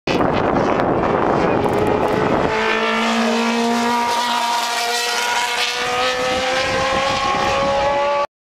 Ronnie Peterson March 721X
Ronnie Peterson's March at Velodromloppet 2011. Very very nice indead!